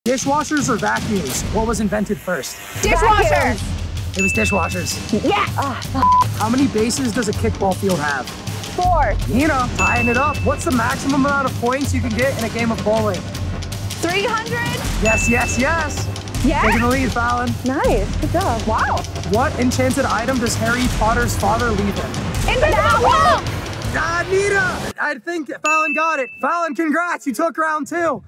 What Came First, Vacuums Or Dishwashers?
Hanging Out | Game Show On A Slackline.